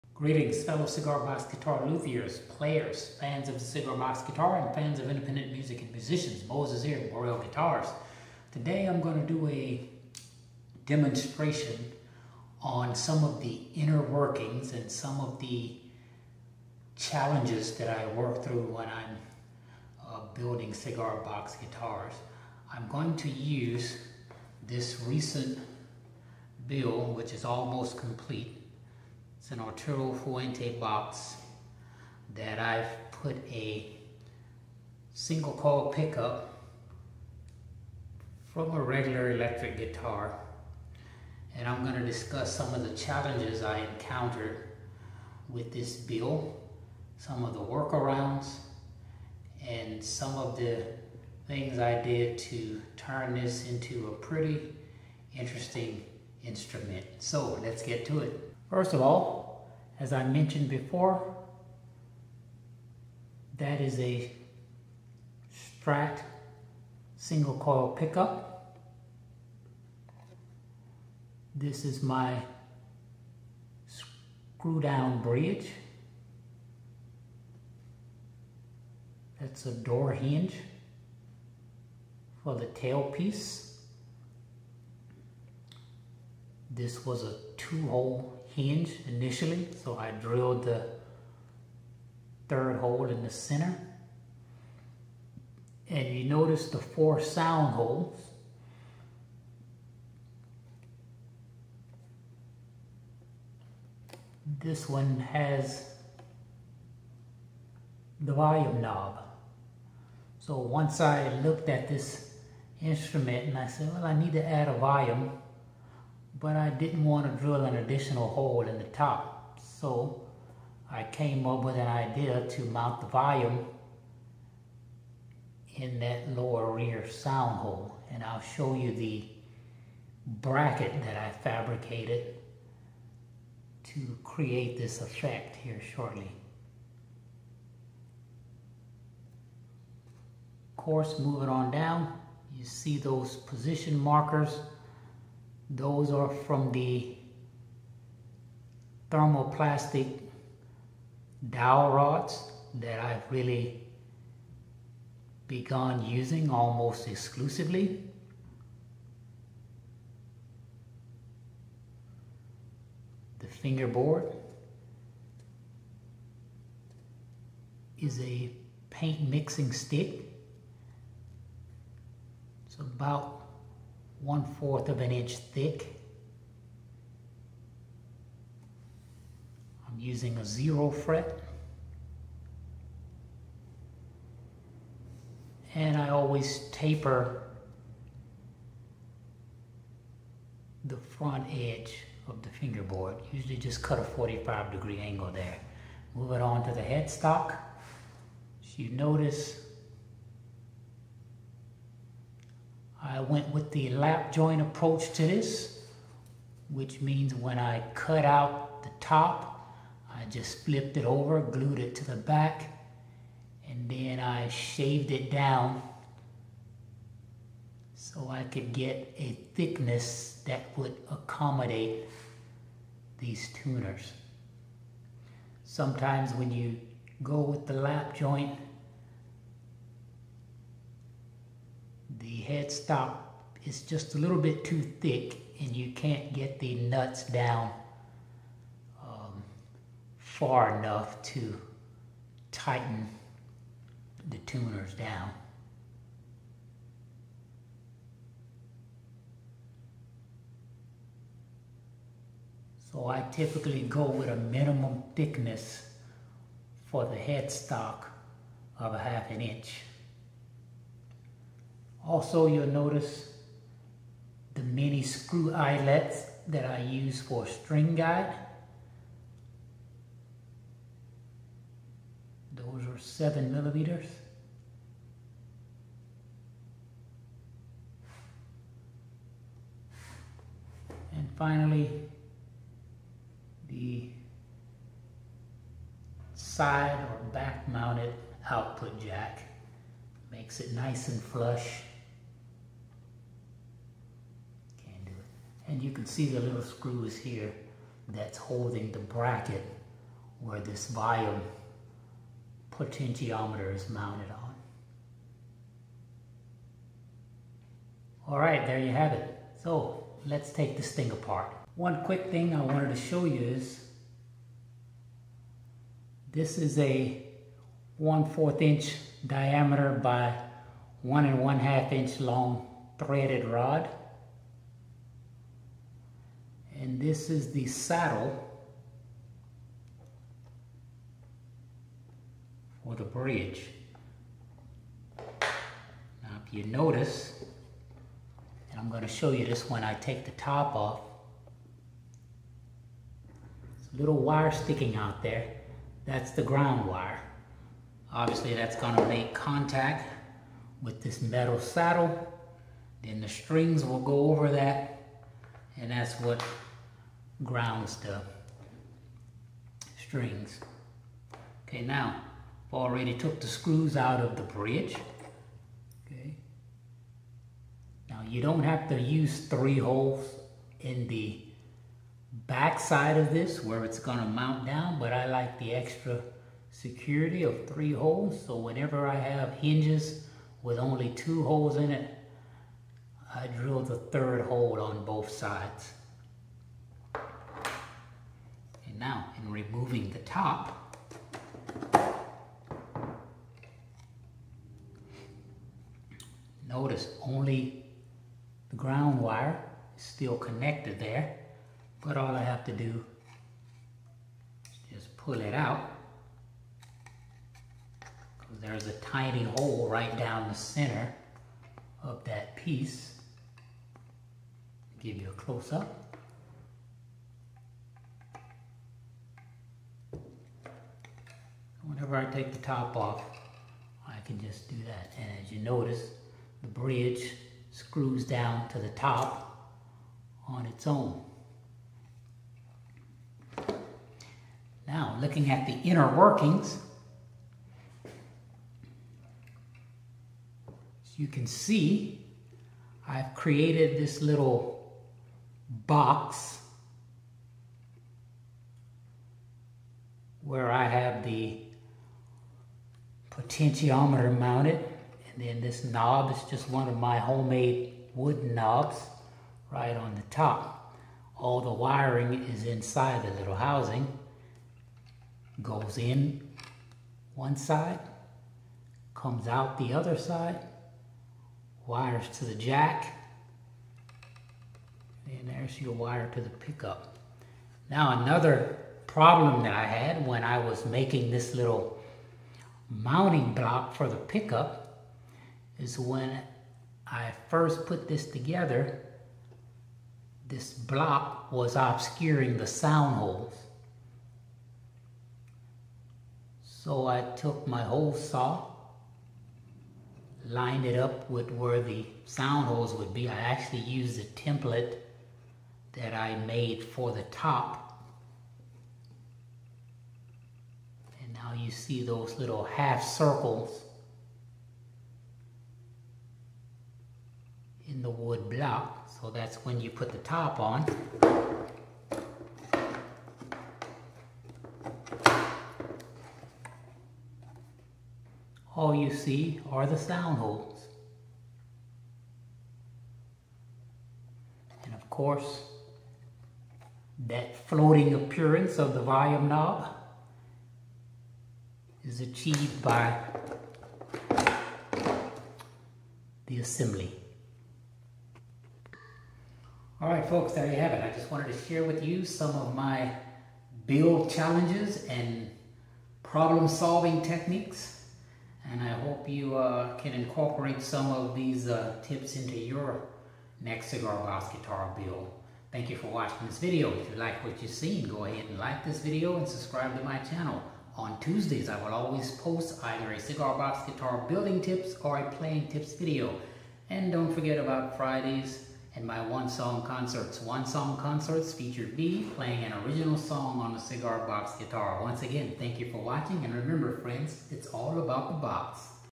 What's Going On Inside This Box?
Building a cigar box guitars can be an exercise in problem-solving.  Here are some of my work-arounds.